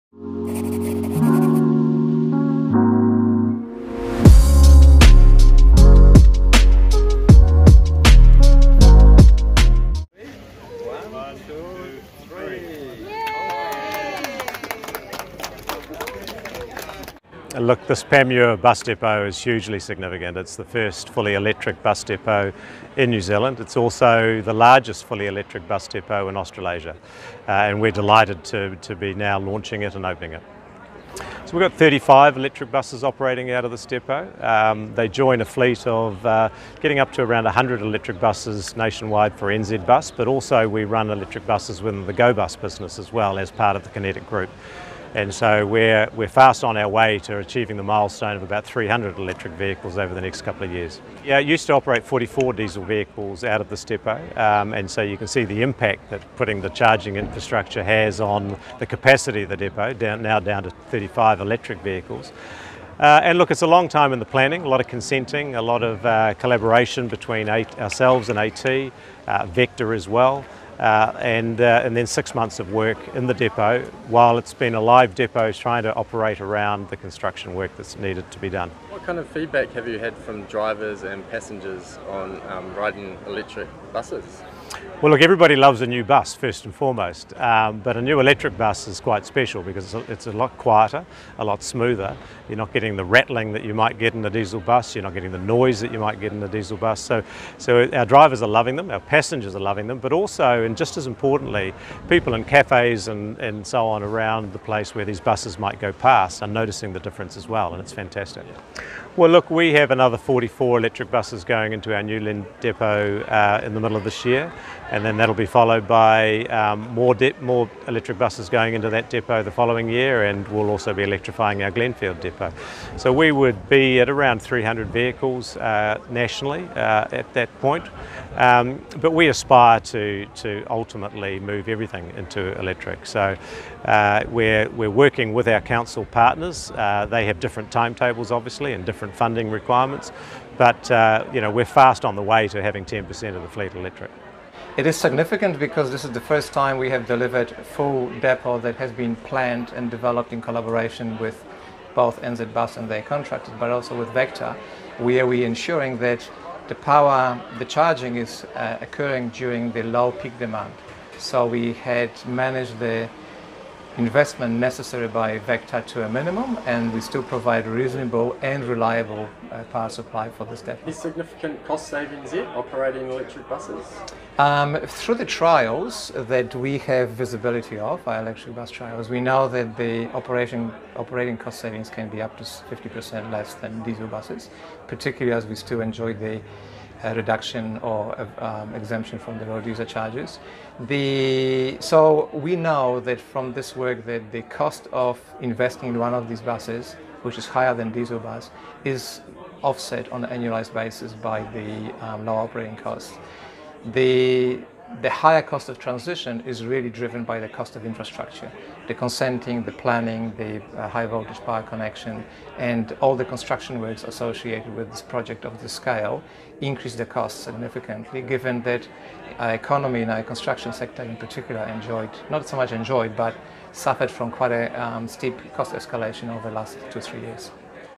New Zealand's first fully electric bus depot unveiled
New Zealand’s first fully electric bus depot has been unveiled in east Auckland and marks largest of its kind in Australasia.